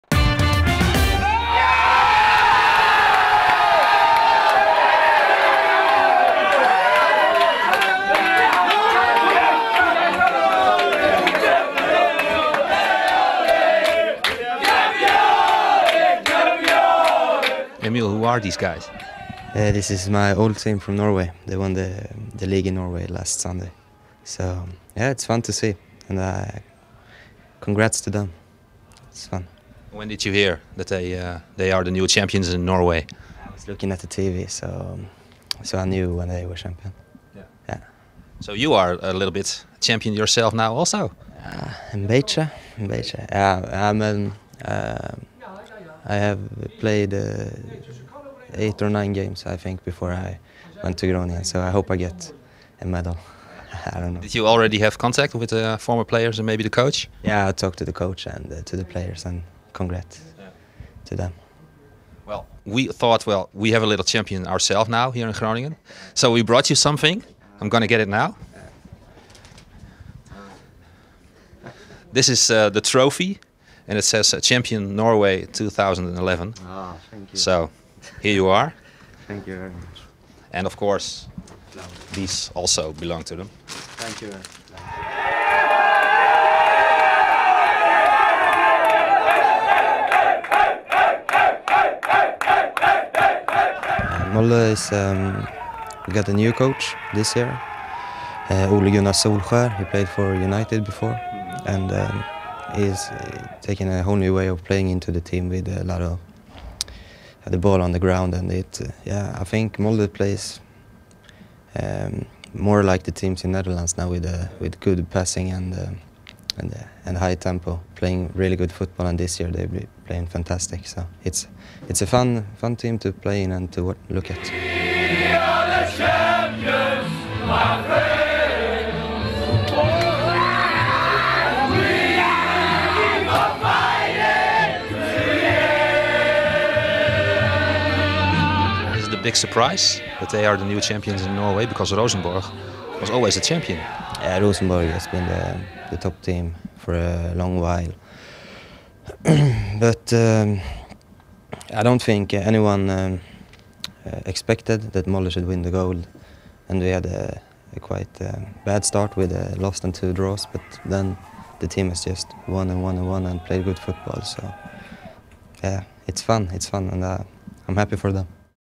Verrassing voor 'kampioen' Emil Johansson
Molde FK, de vorige club van Emil Johansson, werd onlangs kampioen van Noorwegen. Johansson had een aandeel in de verrassende titel omdat hij dit seizoen nog negen wedstrijden voor de ploeg van coach Ole Gunnar Solskjaer speelde. FC Groningen TV zette Johansson daarom in het zonnetje.